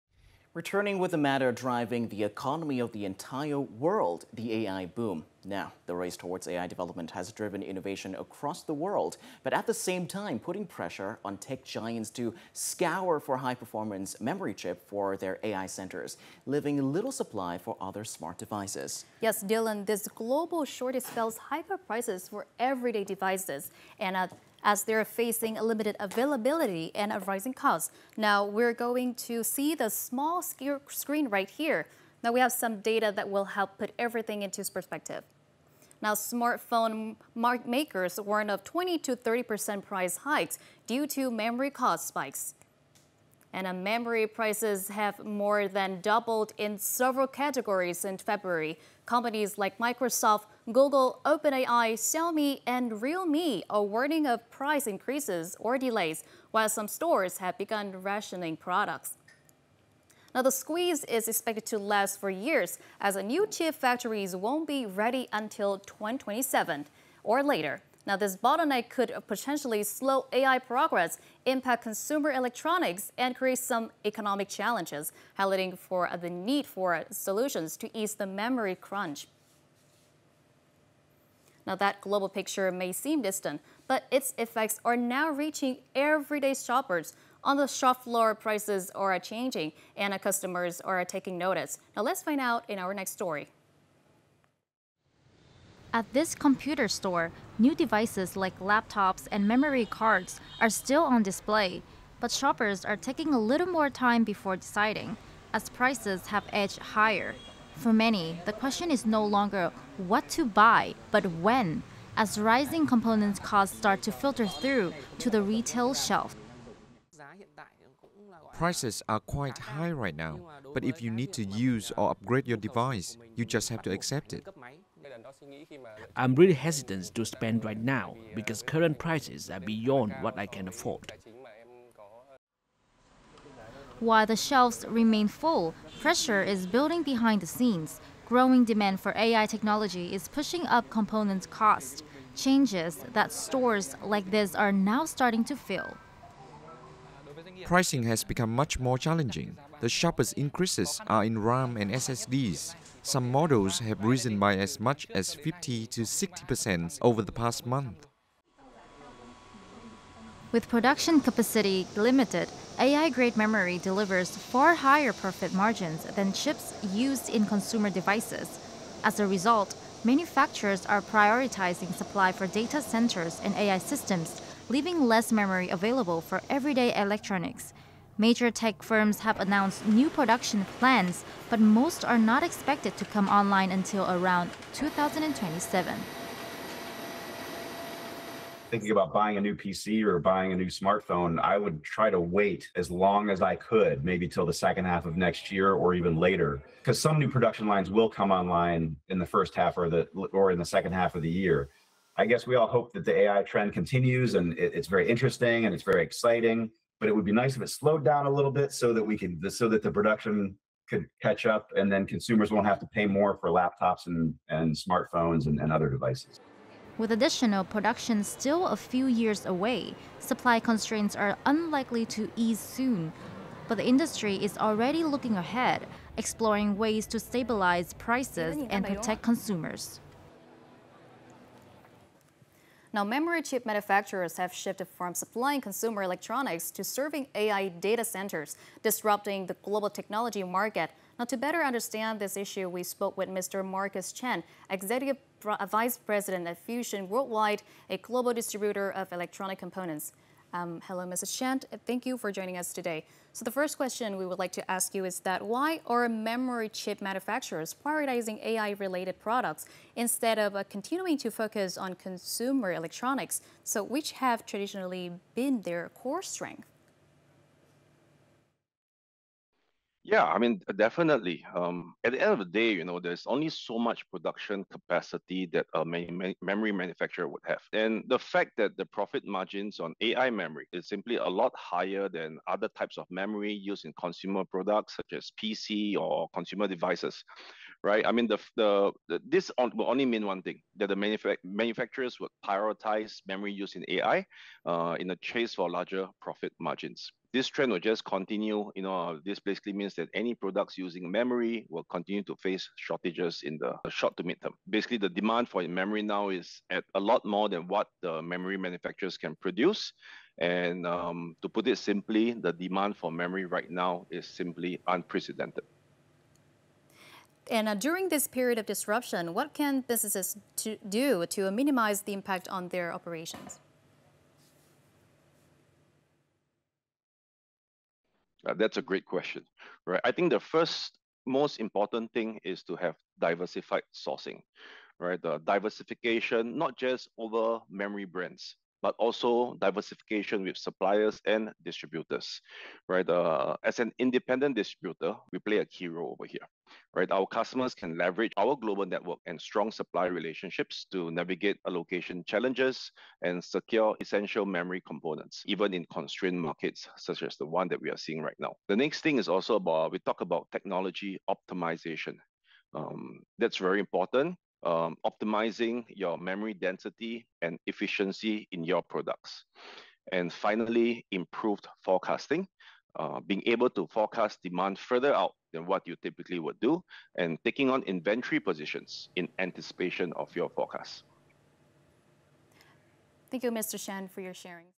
Global memory chip shortage amid AI bloom | Vietnam Today
The race towards AI development has driven innovation across the world, but at the same time, putting pressure on tech giants to scour for high-performance memory chips for their AI centres. leaving little supply for other smart devices.

Or find the Vietnam Today app on Google Play or the App Store.